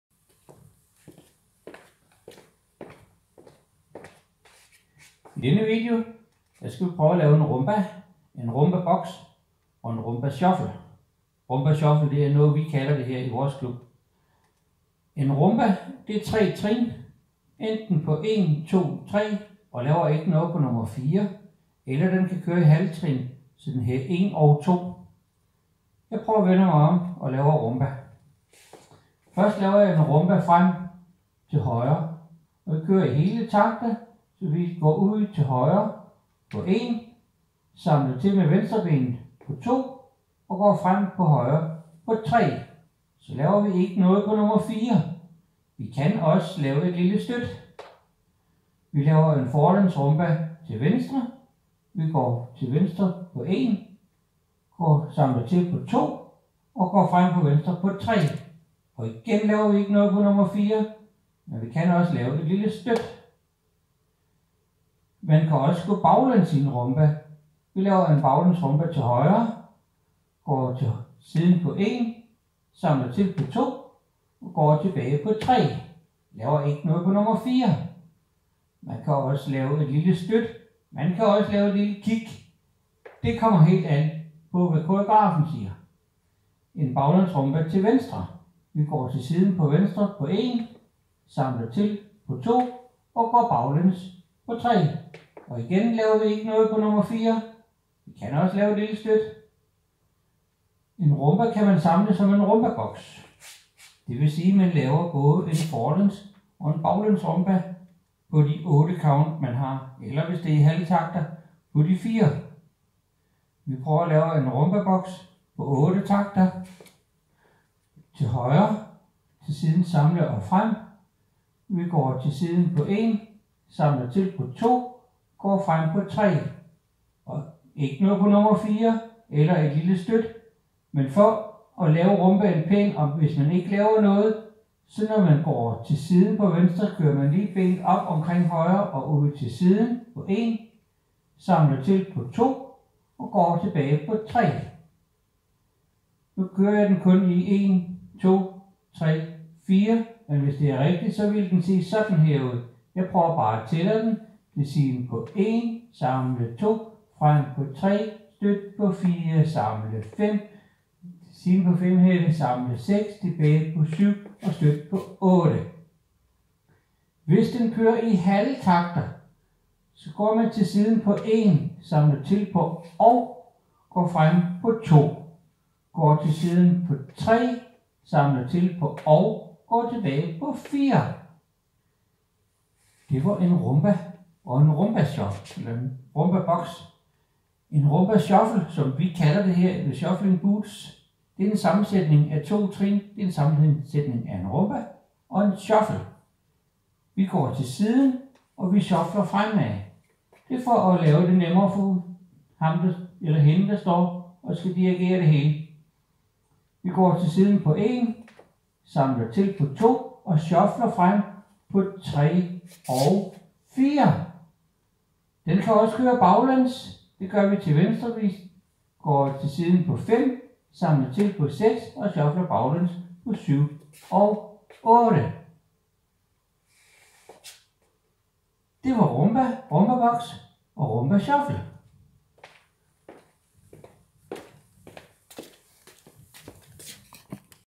Rumba, Rumbabox, Rumbashuffle
Linedance grundtrin forklaret på Dansk.
I denne video gennemgår jeg en Rumba, en Rumbabox og en Rumbashuffle.